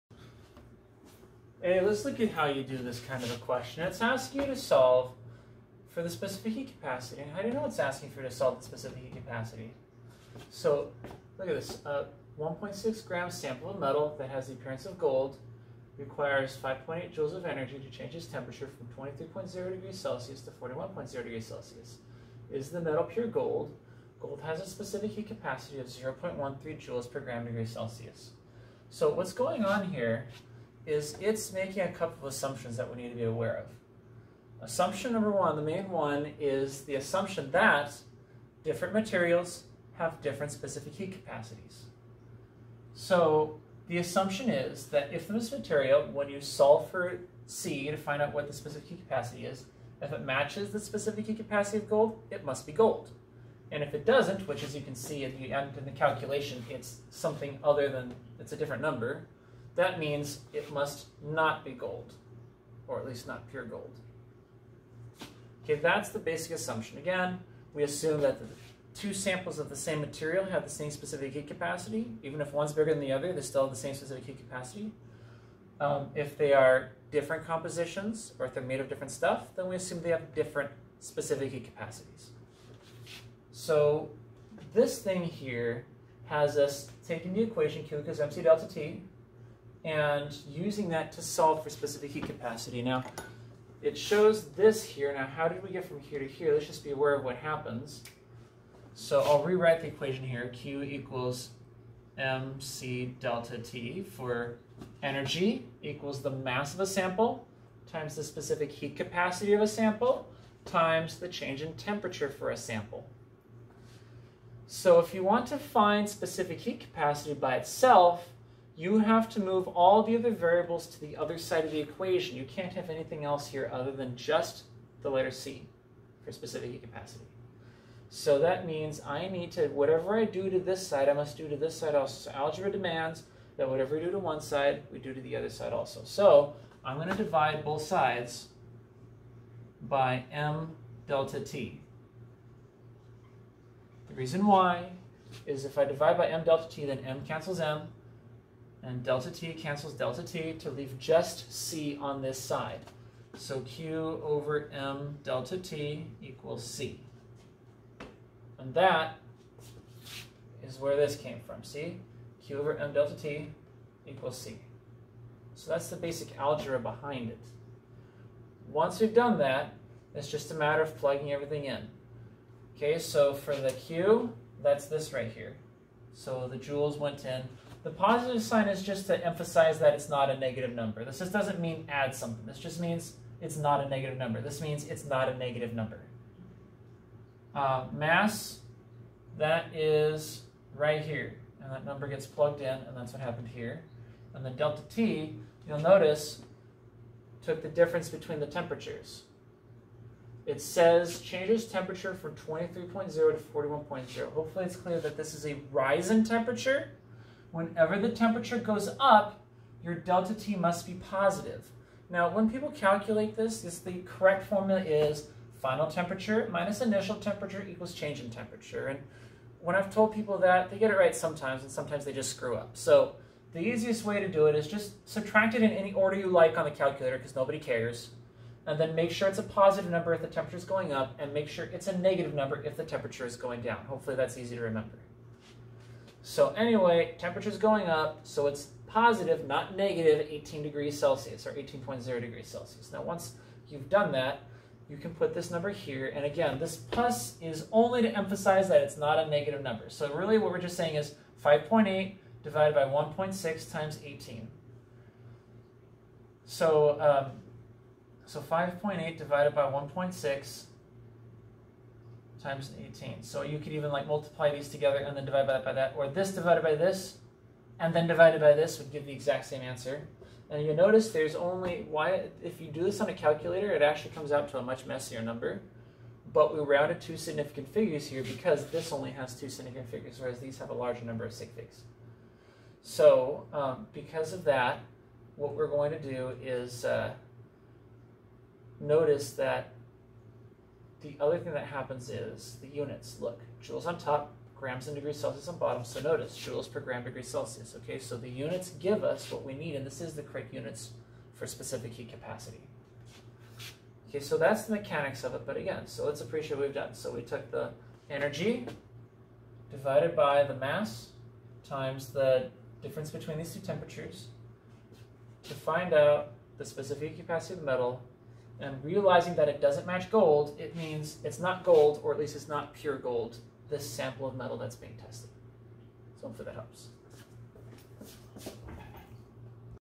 Solving for Specific Heat Capacity
How to interpret the problem and algebraically rearrange the equation algebraically in order to solve for the desired variable